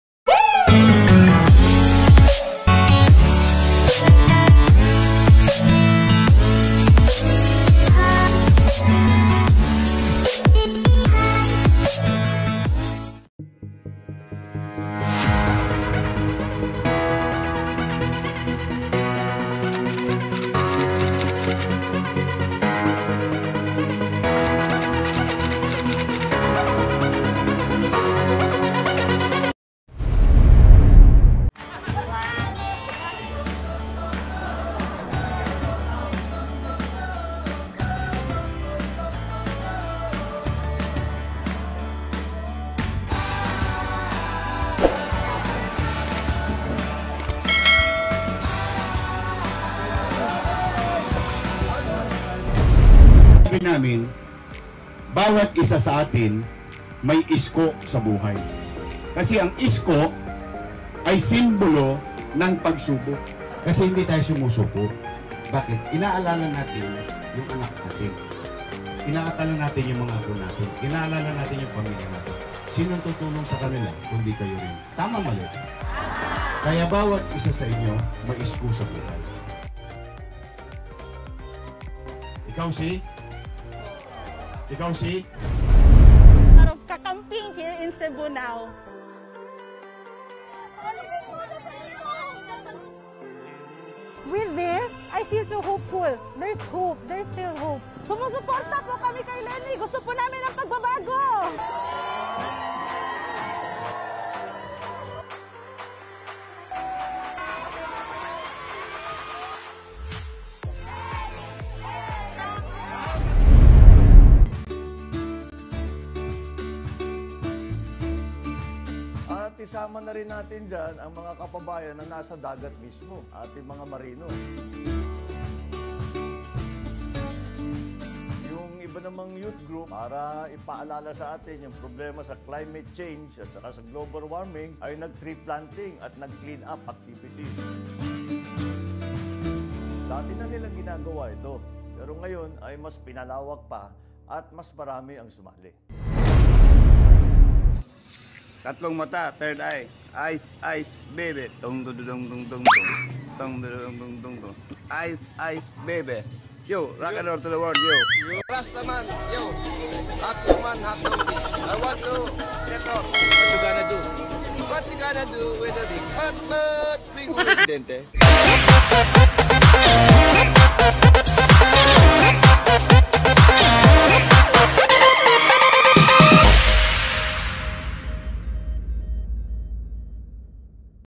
PRESIDENTIAL CANDIDATES 2022 | VOTE WISELY
PRESIDENTIAL CANDIDATES ELECTION OF MAY 2022.

I  hope you enjoy this my upload videos.
Have a good one and god bless!

BANGON PILIPINAS

Kilalanin ang mga tatakbong presidents ngayung 2022.

Sino nga ba  talaga ang nararapat
ngayung mamuno sa ating bansa..

Bangon pilipinas..

Topics.
Manny pacquiao
Isko moreno
Bongbong Marcos
Leno rebredo
Rastaman